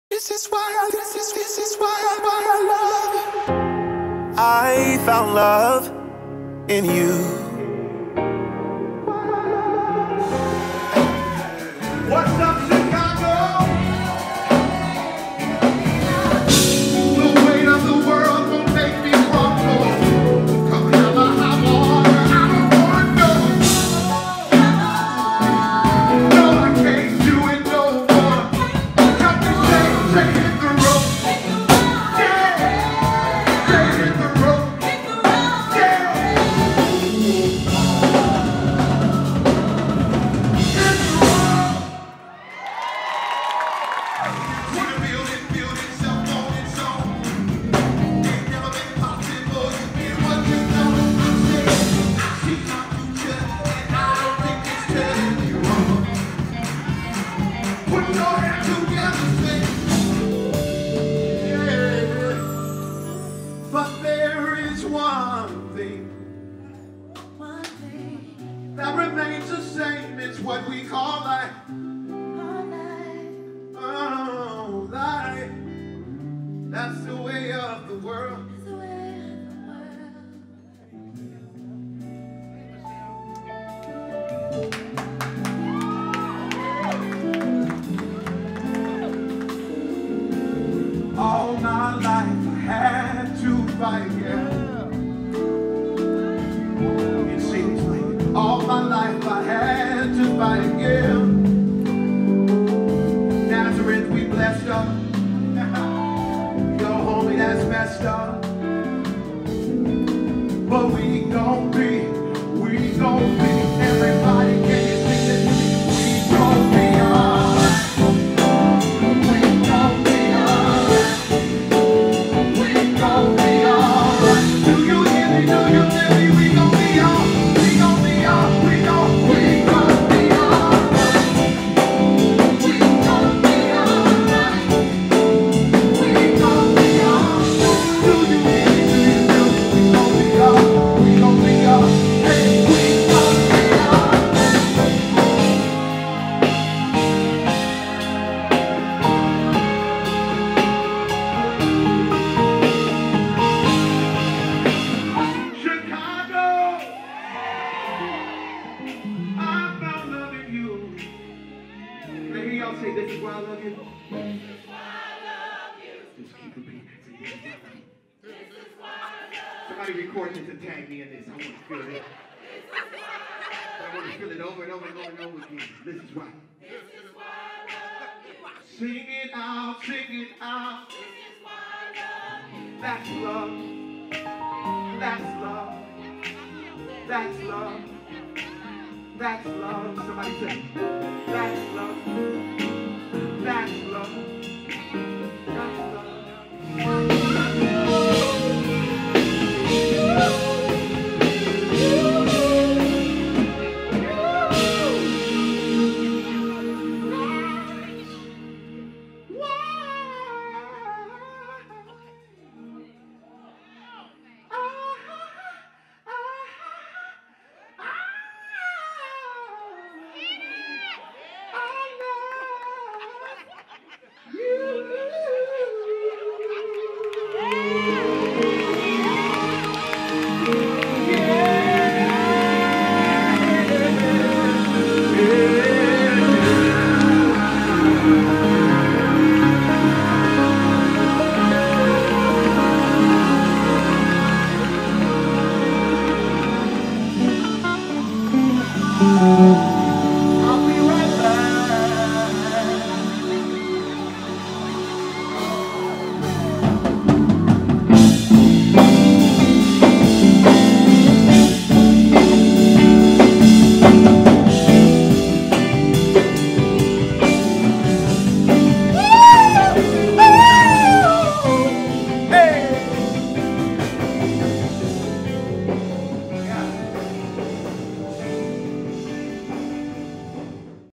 Chicago City Winery Highlights
MrWhyILoveYou, MAJOR., killed the stage this week to the City Winery in Chicago. MAJOR. is one of the best new artists out on the scene and has so much star quality.